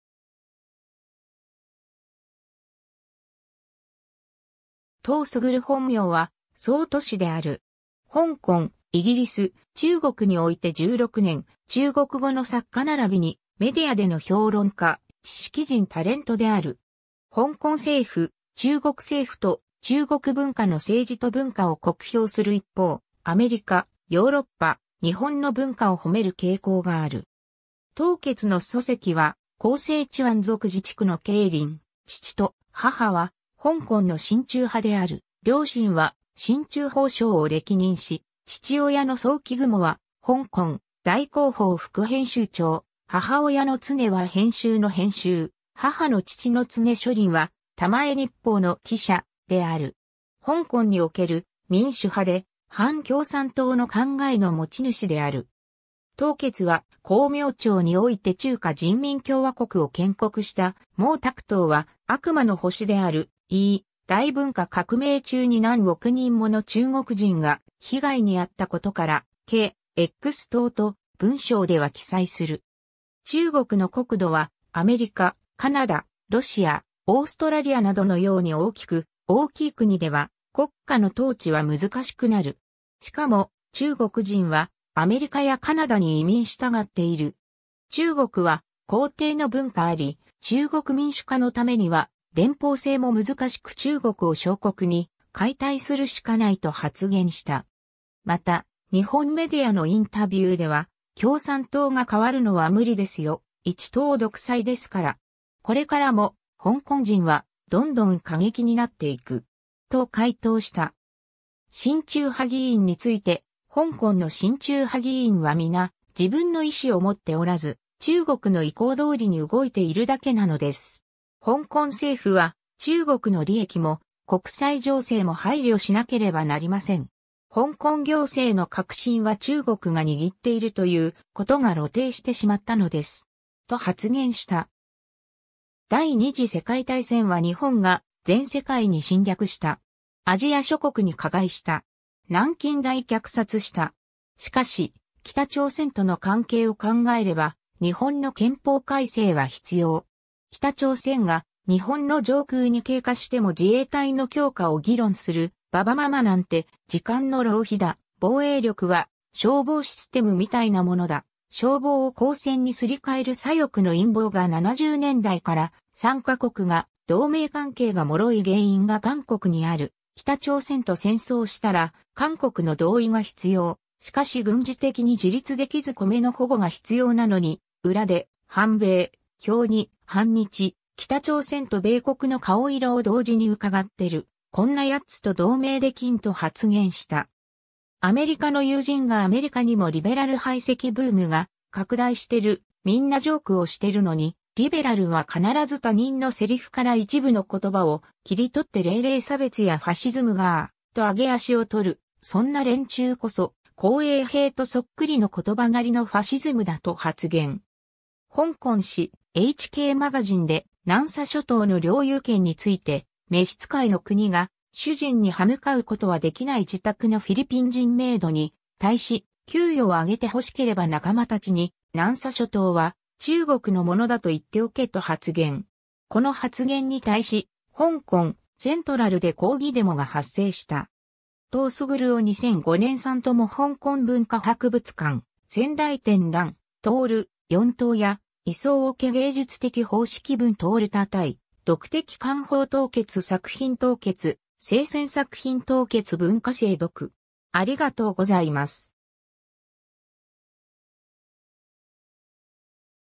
陶傑
陶 傑（とう けつ、英語: Chip Tsao、チップ・ツァオ、1958年8月17日 - ）本名は曹捷である。
香港、イギリス、中国において16年、中国語の作家ならびに、メディアでの評論家、知識人タレントである。
香港政府、中国政府と中国文化の政治と文化を酷評する一方、アメリカ、ヨーロッパ、日本の文化をほめる傾向がある。
 陶傑の祖籍は広西チワン族自治区の桂林、父と母は香港の親中派である、両親は親中報章を歴任し、父親の曹驥雲は香港「大公報」副編集長、母親の常婷婷は編輯の編集、母の父の常書林は「珠江日報」の記者、である。
 香港における民主派で反共産党の考えの持ち主である。
 陶傑は「光明頂」において中華人民共和国を建国した毛沢東は悪魔の星であるいい、大文化革命中に何億人もの中国人が被害にあったことから「毛X東」と文章では記載する。
 中国の国土は、アメリカ、カナダ、ロシア、オーストラリアなどのように大きく、大きい国では国家の統治は難しくなる。
しかも、中国人はアメリカやカナダに移民したがっている。
中国は皇帝の文化あり、中国民主化のためには連邦制も難しく中国を小国に解体するしかないと発言した。
 また、日本メディアのインタビューでは「共産党が変わるのは無理ですよ、一党独裁ですから。これからも、香港人はどんどん過激になっていく。」と回答した。
 親中派議員について「香港の新中派議員は皆、自分の意思を持っておらず、中国の意向どおりに動いているだけなのです。香港政府は中国の利益も国際情勢も配慮しなければなりません。香港行政の核心は中国が握っているということが露呈してしまったのです。」と発言した。
 「第二次世界大戦は日本が全世界に侵略した。アジア諸国に加害した。南京大虐殺した。しかし、北朝鮮との関係を考えれば日本の憲法改正は必要。」「北朝鮮が日本の上空に経過しても自衛隊の強化を議論する婆婆媽媽なんて時間の浪費だ」(注:「婆婆媽媽」とは婦人の間のくだらない喧嘩の意味) 「防衛力は消防システムみたいなものだ、消防を好戦にすり替える左翼の陰謀が70年代から」、「3ヶ国が同盟関係が脆い原因が韓国にある、北朝鮮と戦争したら韓国の同意が必要、しかし軍事的に自立できず米の保護が必要なのに裏で反米、表に反日、北朝鮮と米国の顔色を同時に伺ってる、こんなやつと同盟できん」と発言した。
 「アメリカの友人がアメリカにもリベラル排斥ブームが拡大してる、みんなジョークをしてるのに、リベラルは必ず他人のセリフから一部の言葉を切り取って〇〇差別やファシズムがー、と揚げ足を取る、そんな連中こそ紅衛兵とそっくりの言葉狩りのファシズムだ」と発言。
 香港誌「HK magazine」で、南沙（スプラトリー）諸島の領有権について、「召使の国が主人にはむかうことは出来ない」自宅のフィリピン人メイドに対し、「給与を上げてほしければ仲間たちに南沙諸島は中国のものだと言っておけ」と発言。
この発言に対し、香港・セントラルで抗議デモが発生した。
 陶傑於2005年參與香港文化博物館專題展覽「融—四頭家」，以裝置藝術的方式分享他對閱讀的看法 陶傑作品 陶傑精選作品 陶傑文化精讀